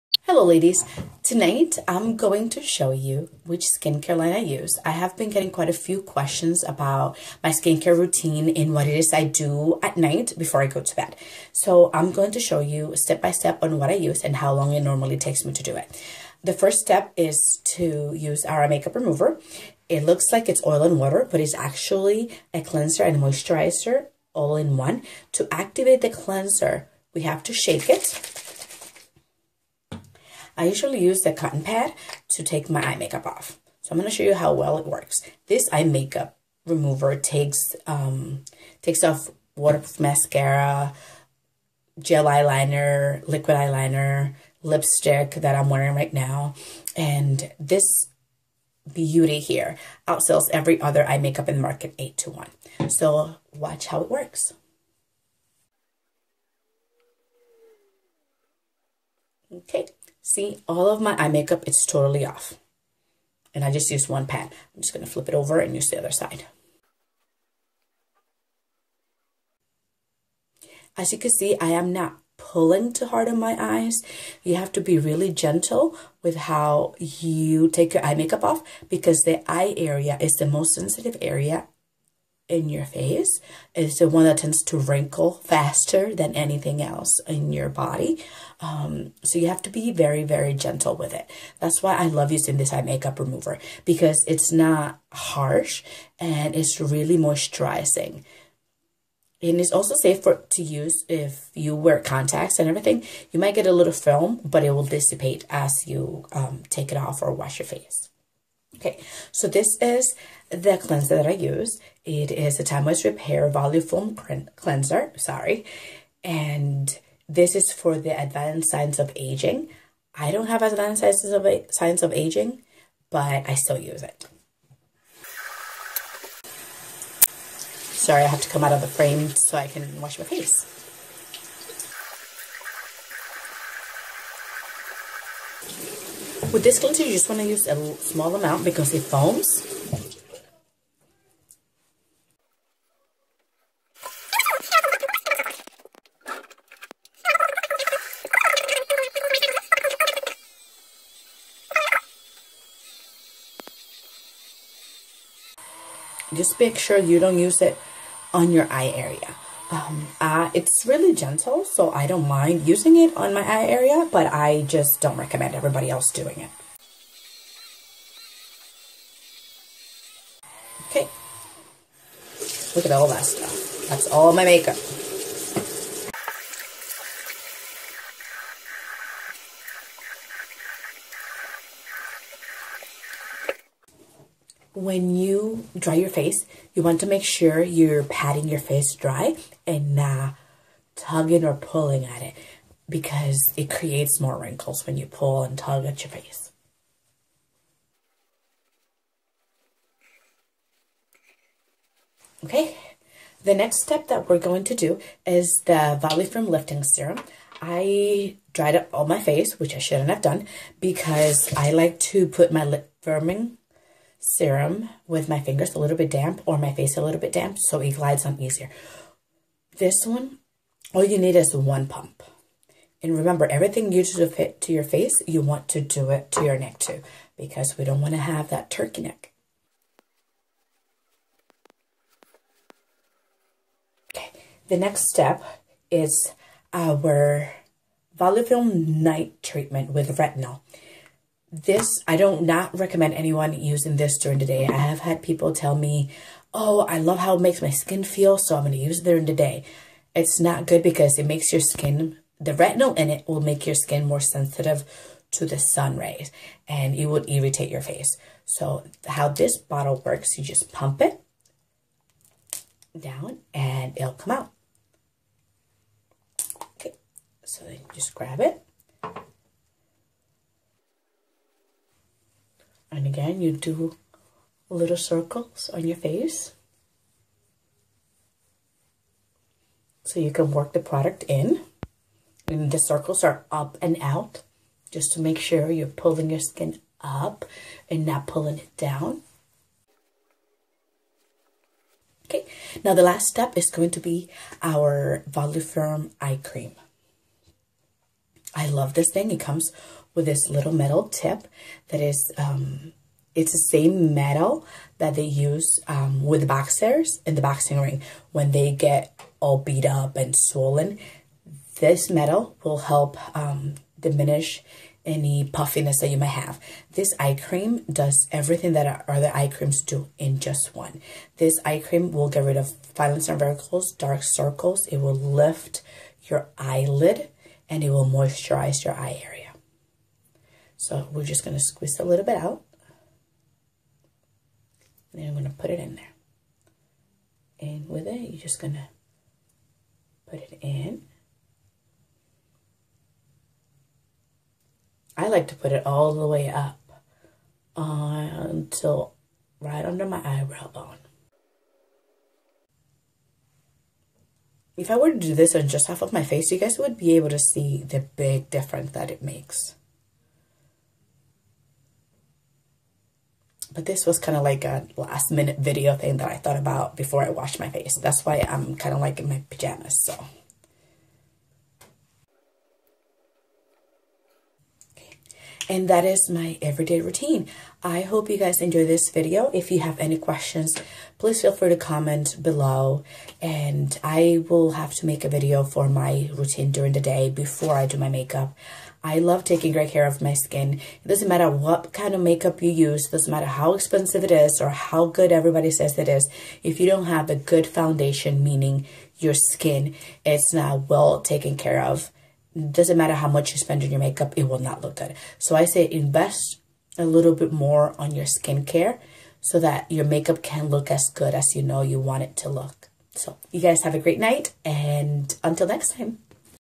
TW Repair night routine (Winter)
I made this video using our Time Wise Repair Skincare Line. I use this line mostly in the winter when my skin is more dry. During the summer I use our Time Wise Miracle Set in Combination to Oily.